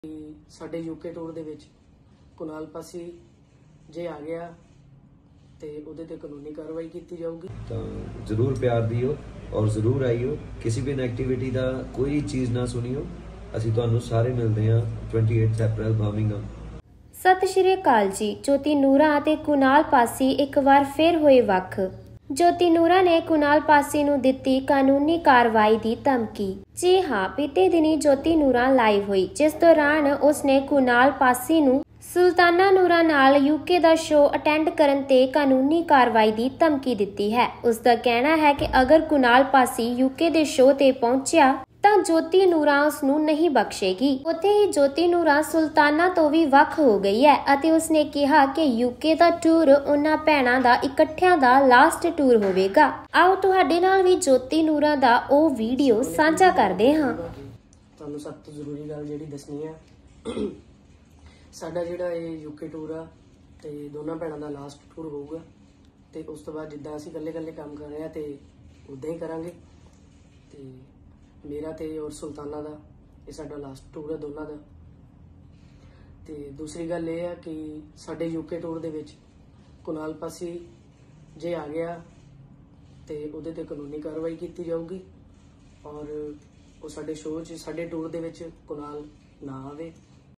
Jyoti Nooran ne husband Kunal Pasi nu ditti Dhamki | Jyoti Nooran Sulatana breakup@TrendyAlert
Jyoti Nooran ne husband Kunal Pasi nu ditti Dhamki | Jyoti Nooran Sulatana breakup